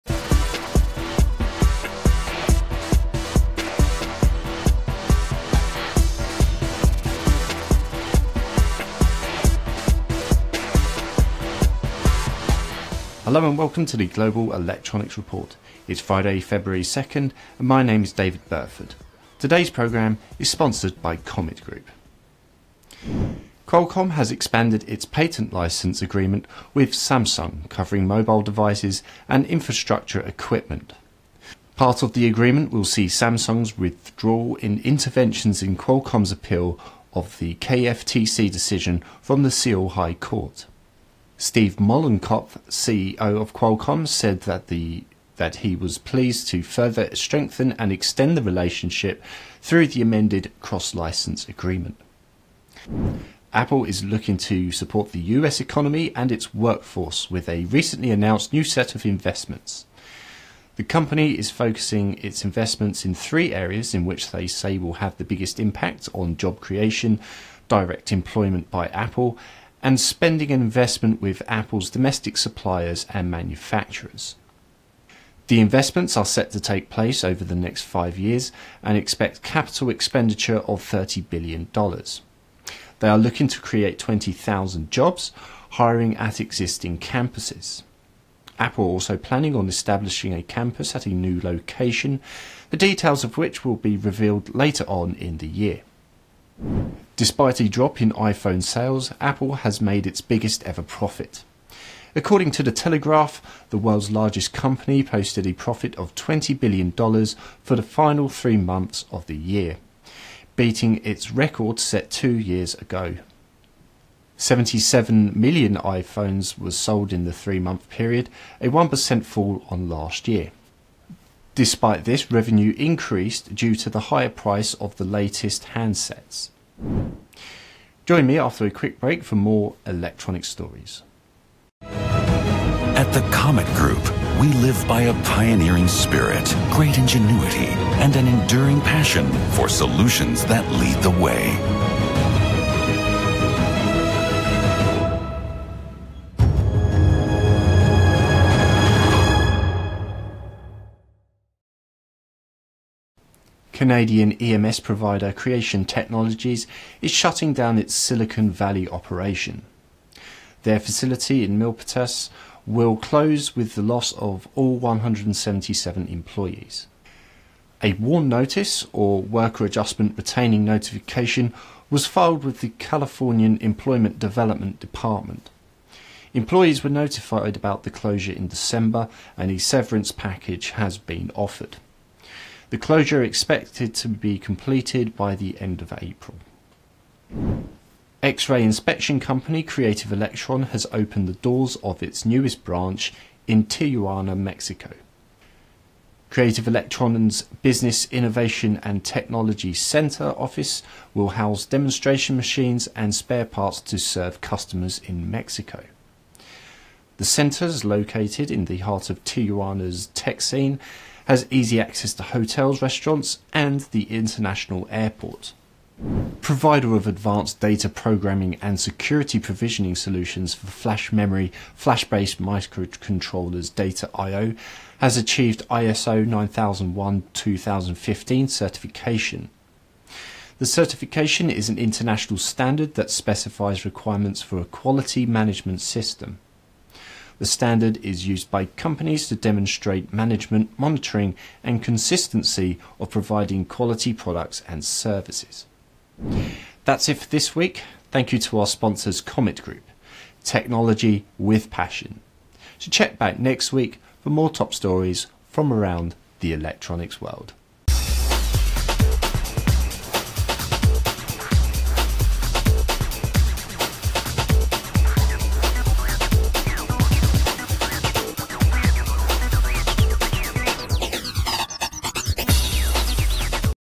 Global SMT & Packaging Electronics Report 02-02-2018
David Burford brings you some of the week's top stories from around the electronics manufacturing industry including Apple’s record profits and investment plans, Creative Electron’s new Mexican facility pus more!